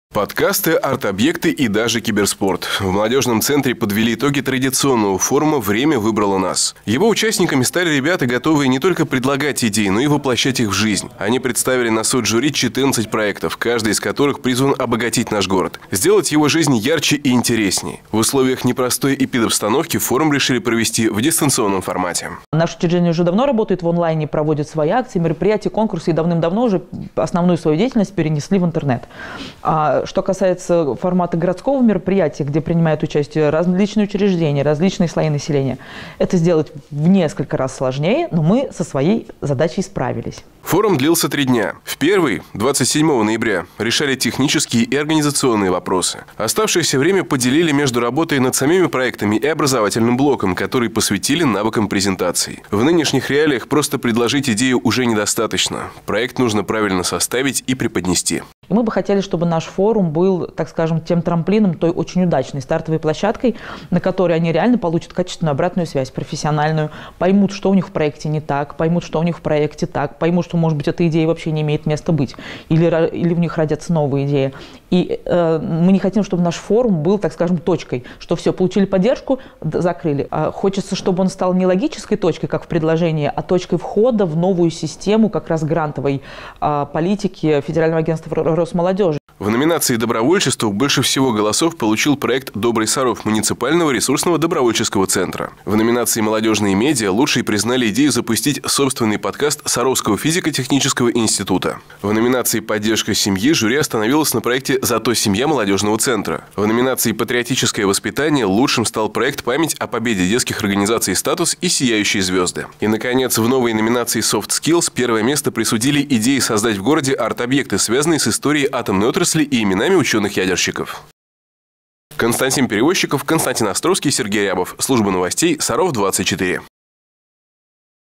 Время выбрало нас
В Сарове прошёл молодежный форум «Время выбрало нас». В этом году из-за ограничений основное действие перенесли в онлайн-формат.